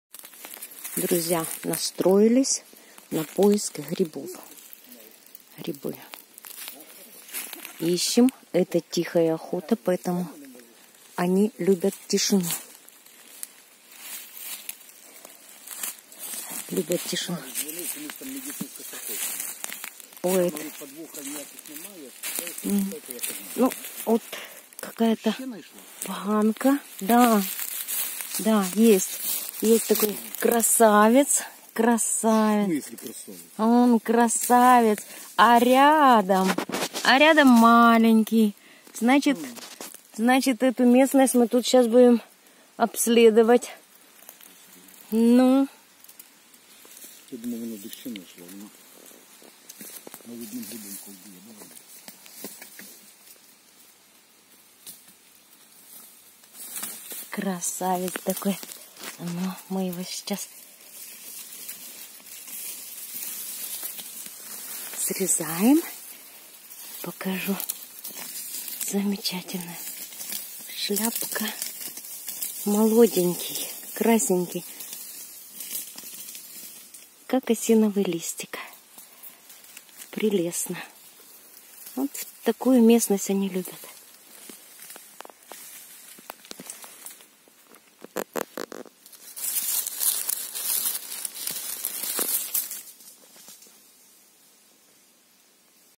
Подосиновики пошли.
Мы нашли подосиновики. Конечно, у нас их ещё мало, только начинаются.